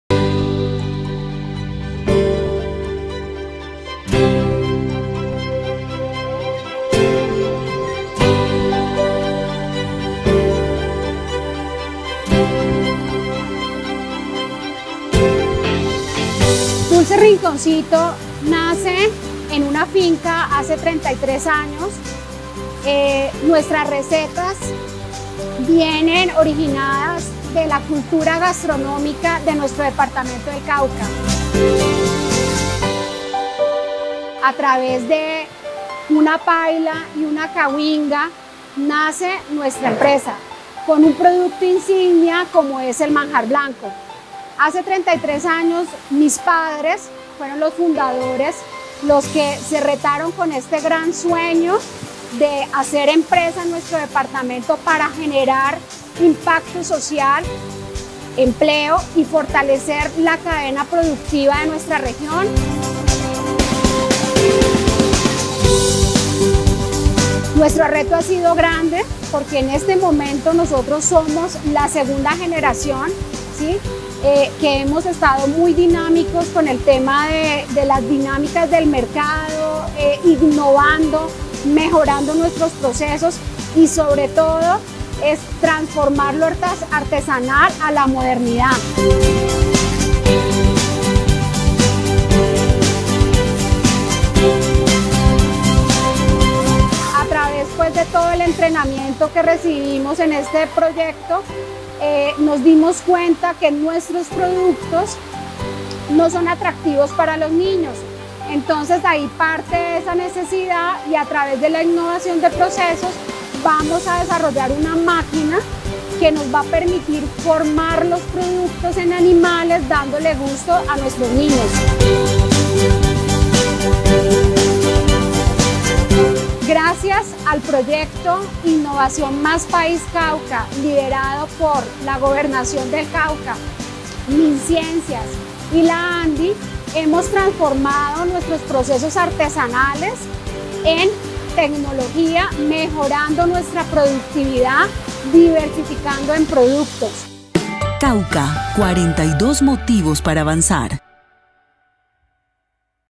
DULCES RINCONCITO, EMPRESA CAUCANA FAVORECIDA DEL PROYECTO INNOVACIÓN MÁS PAÍS
Avanzando 💡| La Gobernación del Cauca en articulación con Minciencias y la ANDI, impulsa la implementación de proyectos innovadores que fortalecerán la competitividad y productividad de las empresas de la región.

🏭 Presentamos a Dulces Rinconcito, empresa caucana favorecida del proyecto Innovación Más País.